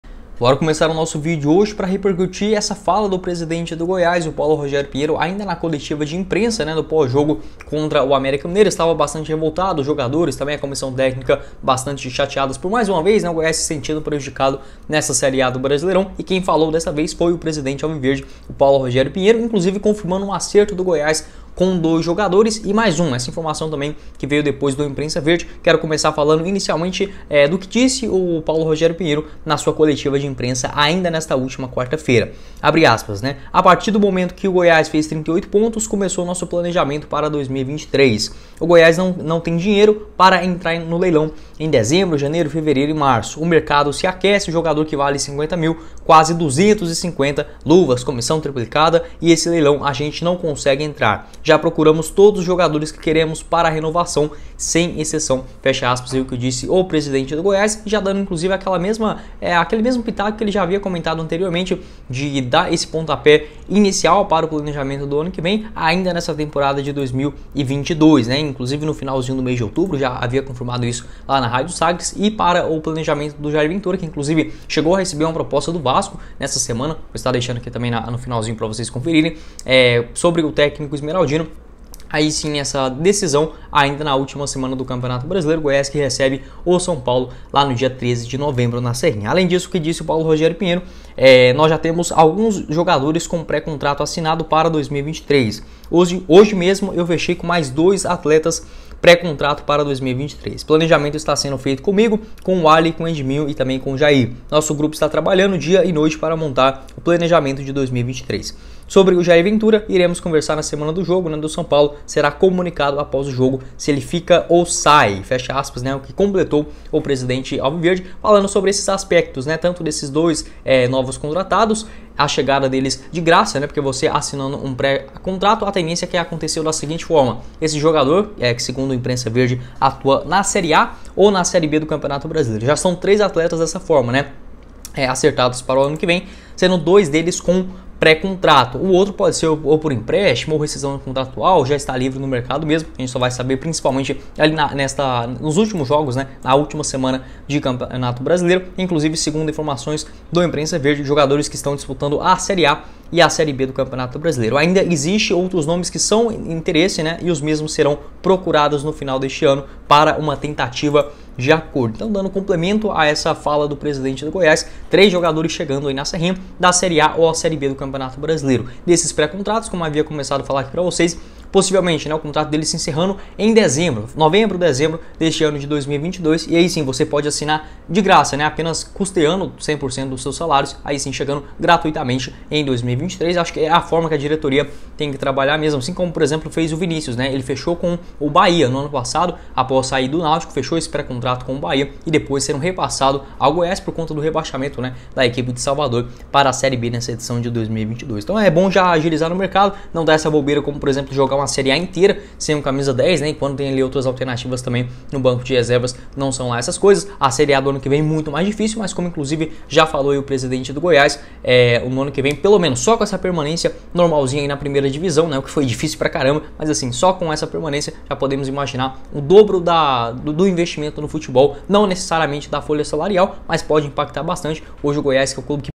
💚😱 FECHADOS! GOIÁS TEM 3 JOGADORES CONTRATADOS PARA 2023!
✅💰 FAÇA JÁ A SUA APOSTA NA SORTE GOLBET